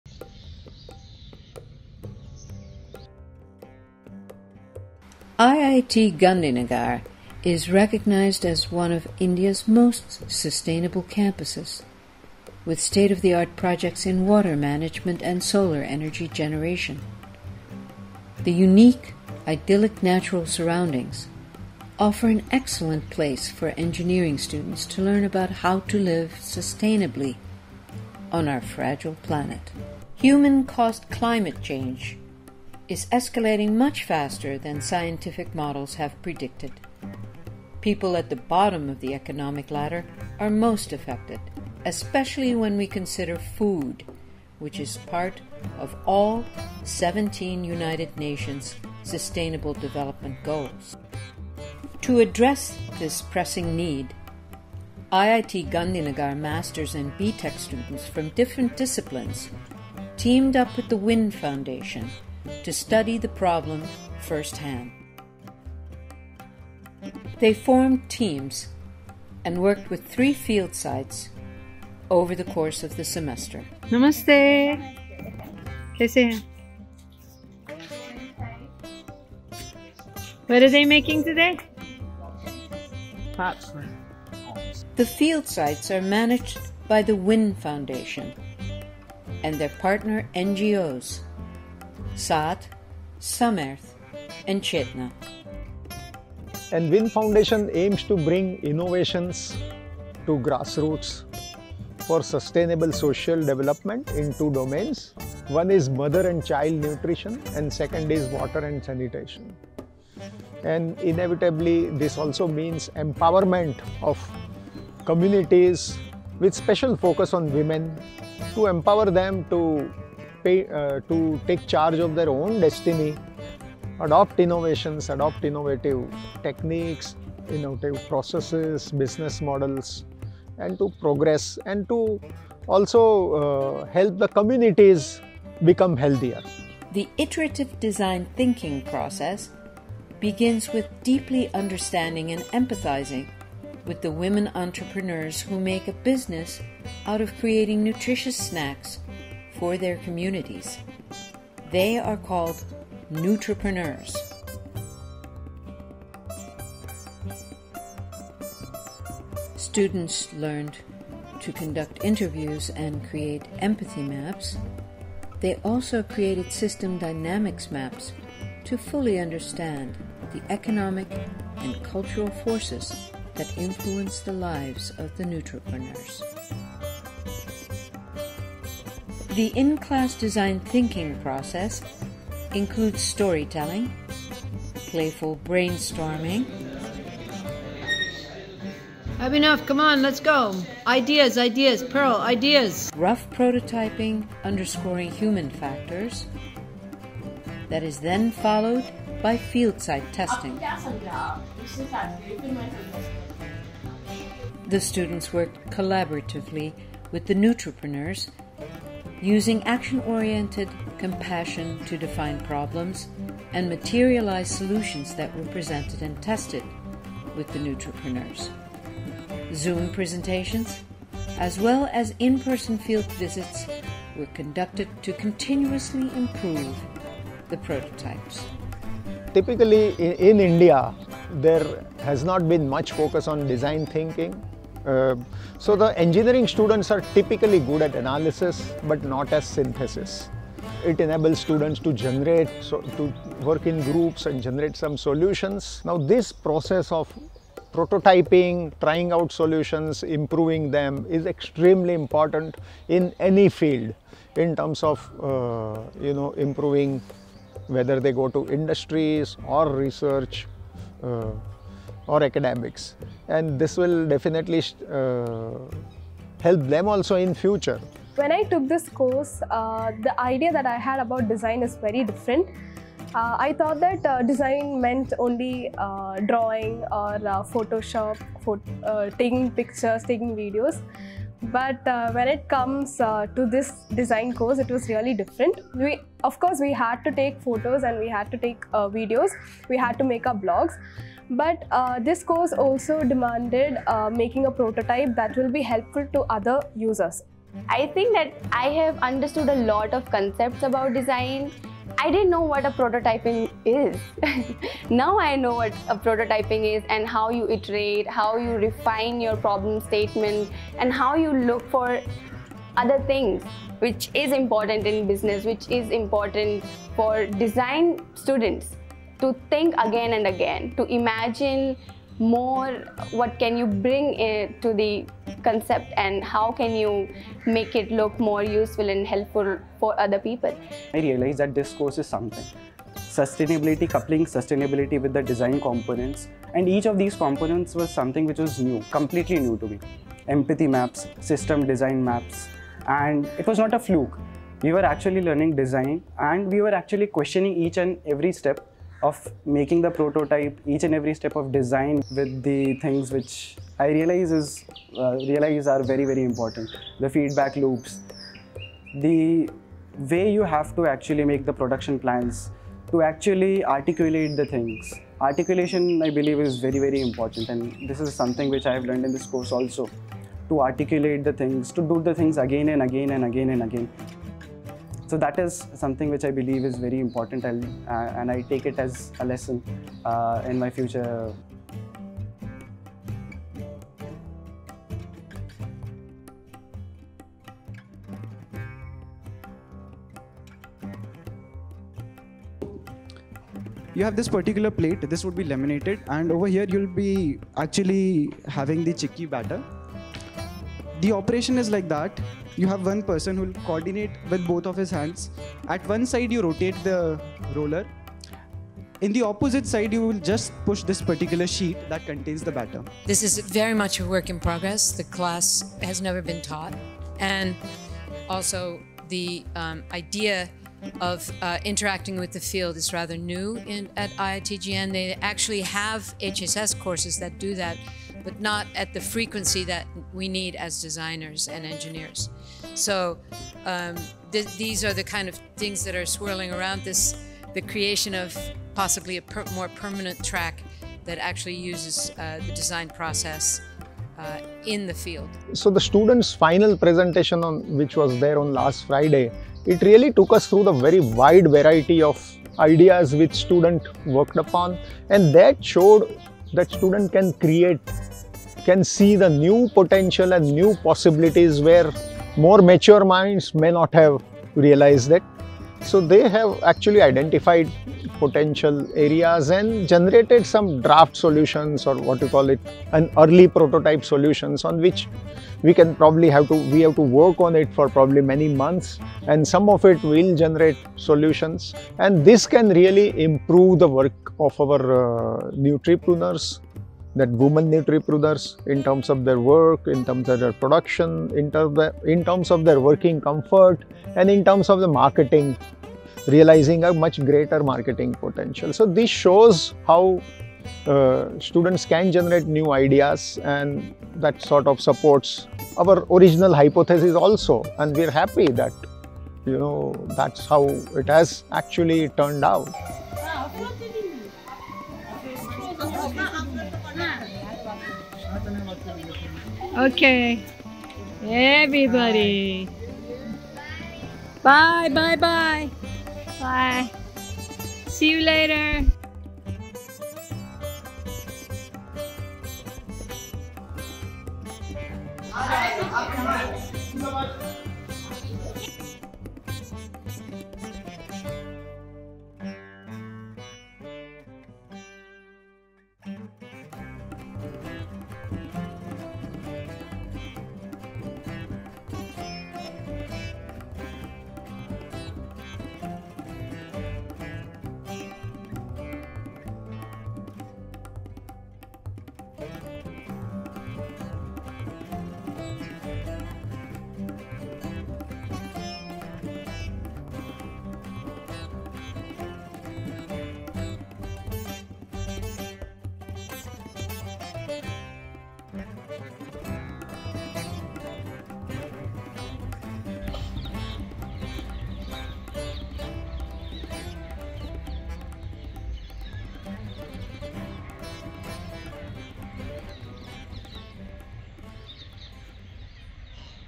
Sustainable Design in Engineering with Human Factors | Prof Anezka Sebek
Sustainable Design in Engineering with Human Factors

This short film showcases Prof Anezka Sebek’s course at IITGN titled ‘Sustainable Design in Engineering with Human Factors’ course, a collaboration with WIN Foundation, SAATH, CHETNA and SAMERTH NGOs and the ‘Nutripreneurs’- women who are creating businesses to make nutritious snacks for children and their neighbours.

Prof Anezka Sebek
Visiting Faculty